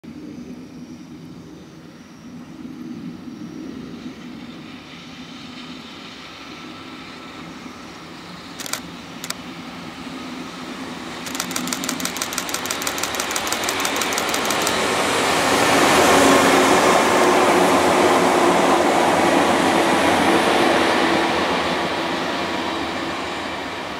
【赤電塗装】伊豆箱根鉄道 大雄山線 5000系 甲種輸送【EF65-2130牽引・HM付】
9月21日、大場工場を出場した伊豆箱根鉄道5000系5501編成が三島→相模貨物で甲種輸送されました。

2016/09/21 JR東海道線 二宮～大磯